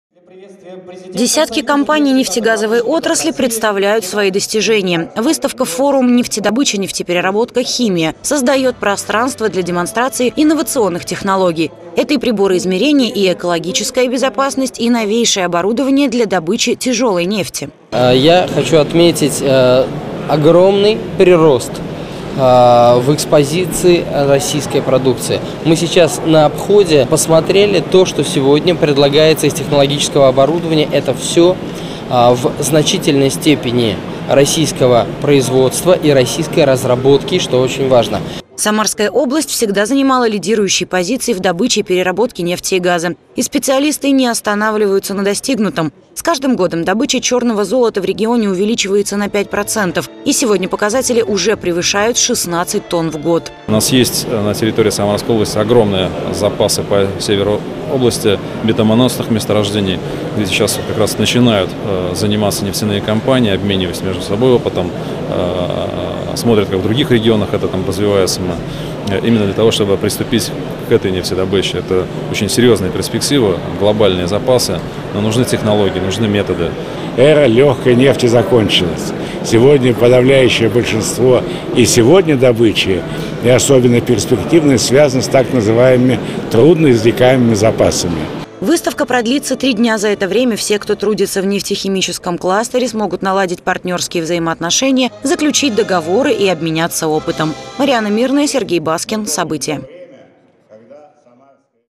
Специалисты нефтяного кластера вновь представляют свои достижения.
Они знают все о чёрном золоте. Специалисты нефтяного кластера вновь представляют свои достижения. Десятая юбилейная выставка - Нефтедобыча, Нефтепереработка, Химия открылась в выставочном комплексе Экспо-Волга.